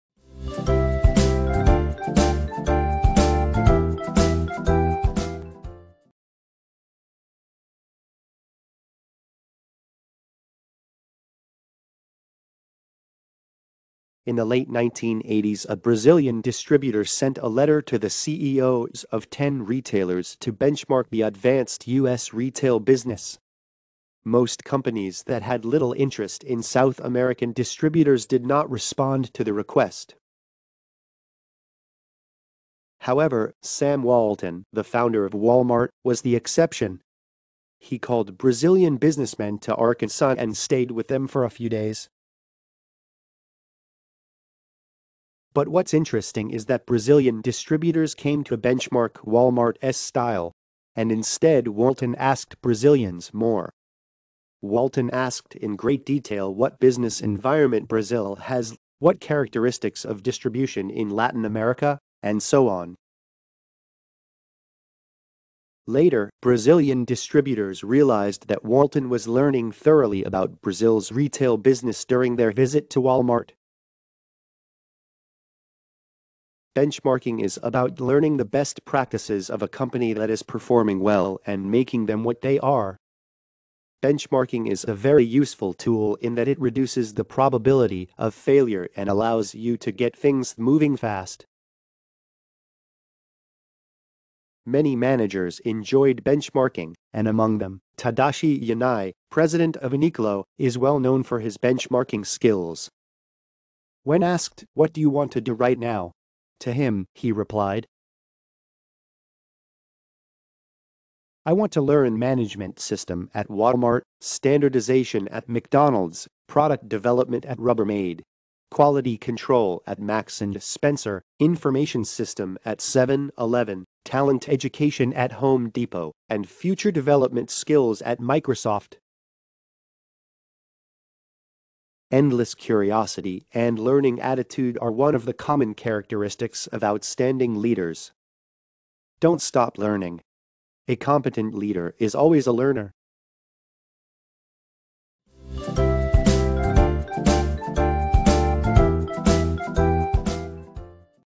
Short Story: A competent leader is always a learner
Endless curiosity and learning attitude are one of the common characteristics of outstanding leaders. Don't stop learning. A competent leader is always a learner.